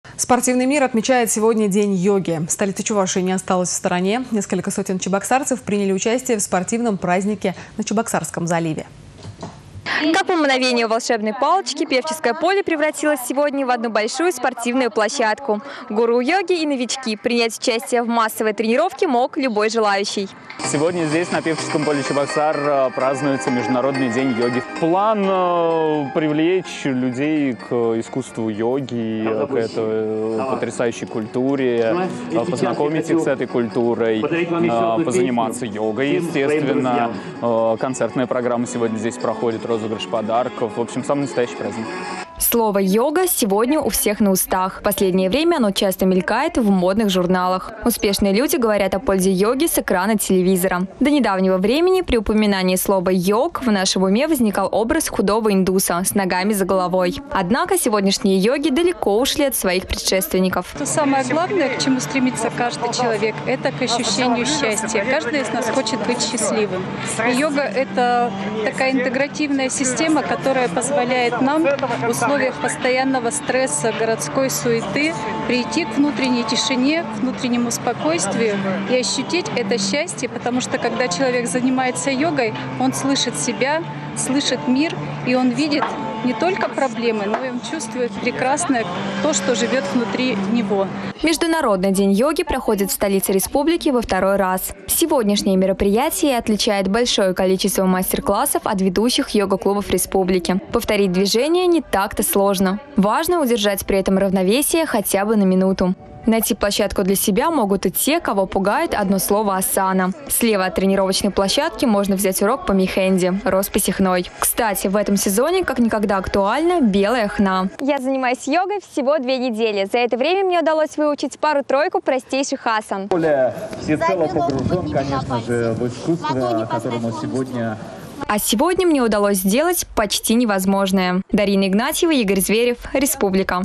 Международный день йоги
Спортивный мир отмечает сегодня день йоги. Столица Чувашии не осталась в стороне. Несколько сотен чебоксарцев приняли участие в спортивном празднике на Чебоксарском заливе.